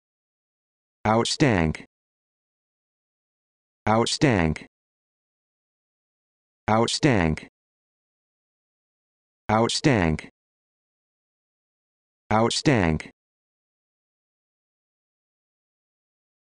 How to Pronounce Outstank
This video shows you how to pronounce Outstank